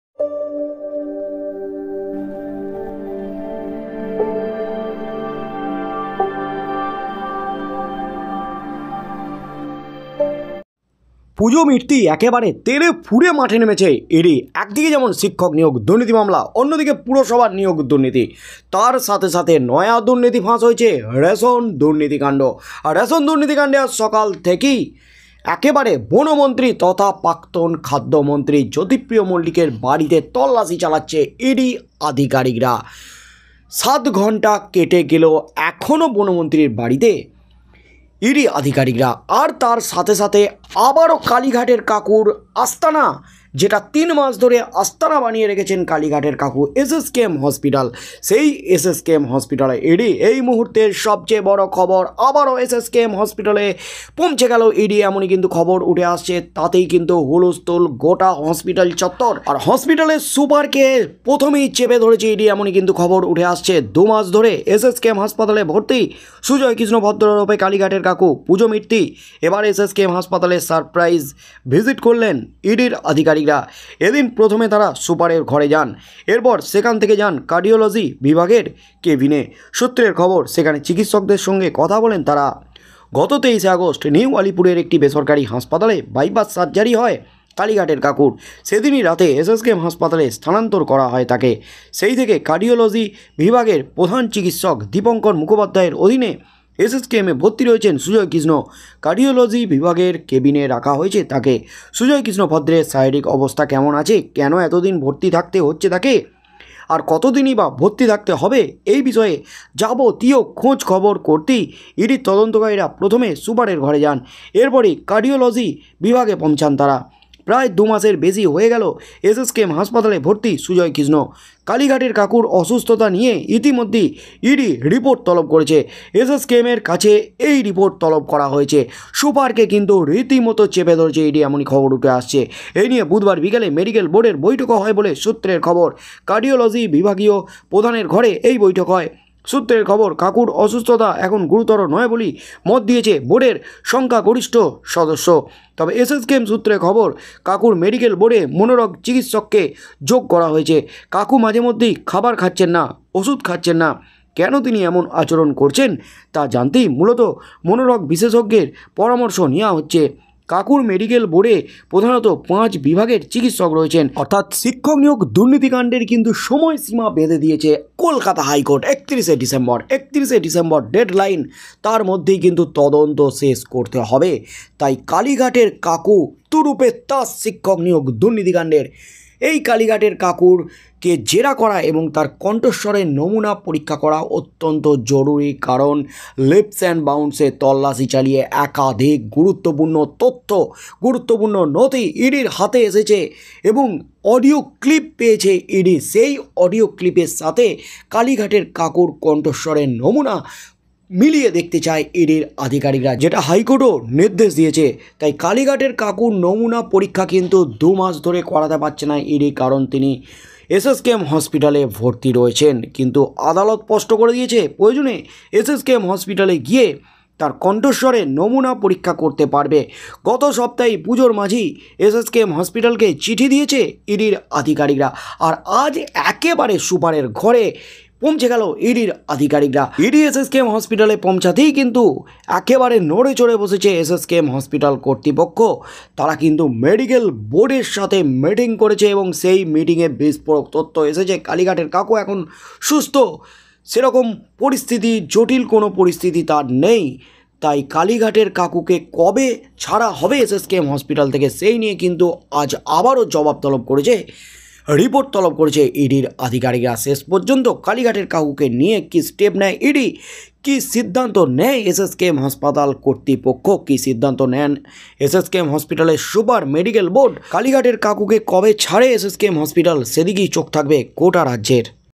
SSKM এ ইডির হানা! হসপিটালের সুপারকে চেপে ধরতেই কাকুর আসল মেডিকেল রিপোর্ট ফাঁস! কাঁপুনি শুরু কাকুর
ধন্যবাদ

Keyword⏩